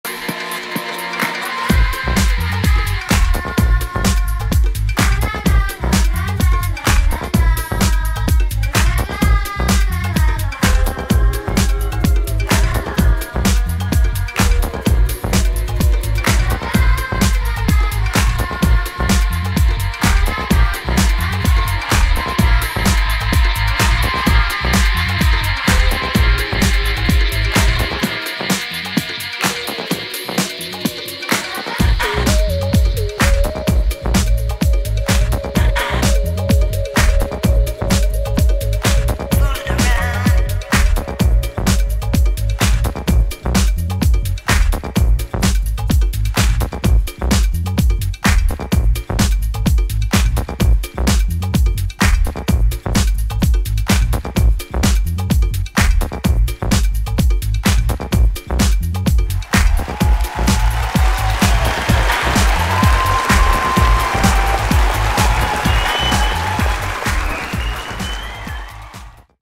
Crew Savage feat. Cuartero
Aftermovie de la presentación de Cuartero en Crew Savage el 24 de junio de 2022. Palacio Alsina - Buenos Aires - Argentina.